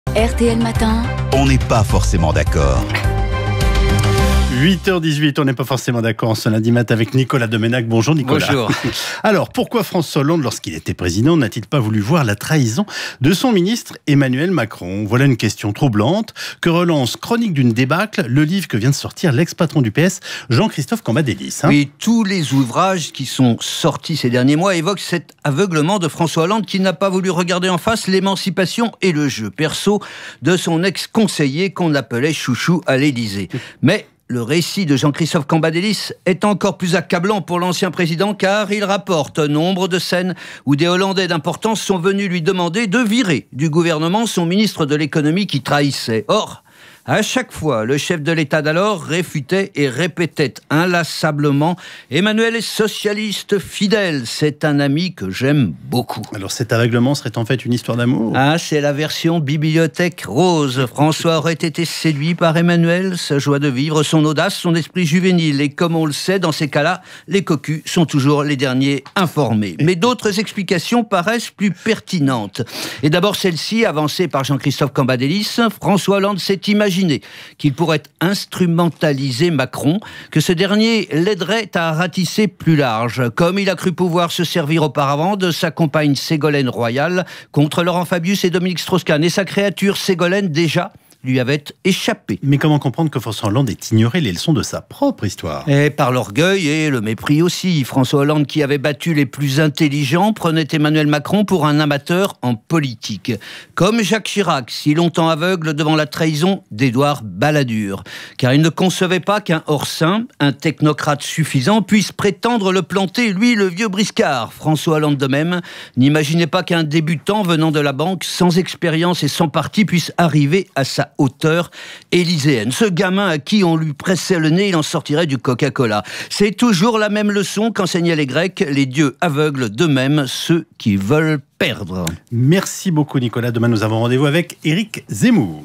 Domenach : "Pourquoi Hollande n'a pas voulu voir la trahison de Macron"
Dans un livre, Jean-Christophe Cambadélis rappelle que l''ancien chef de l'État n'avait pas vu non plus la candidature de Royal en 2017.